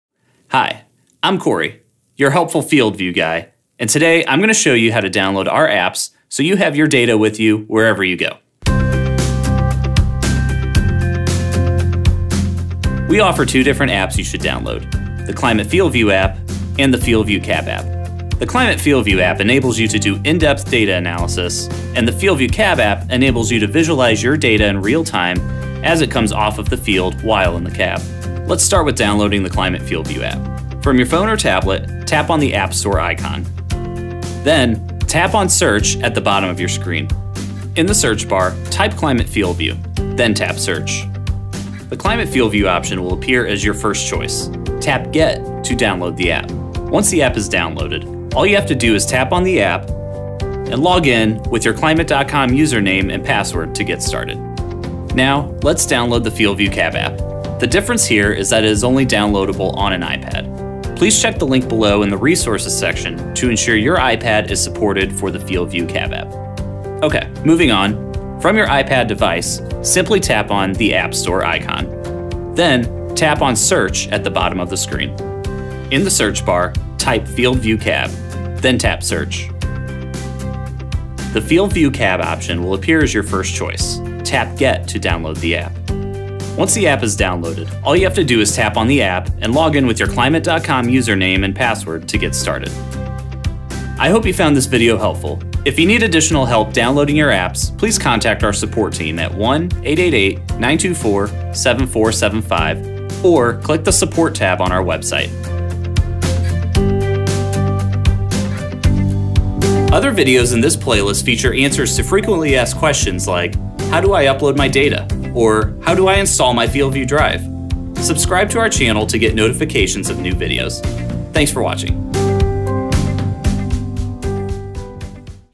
FieldView 101: Downloading Apps
Your helpful FieldView guy, Corey, walks you step-by-step how to download our apps to take your farming data wherever you go.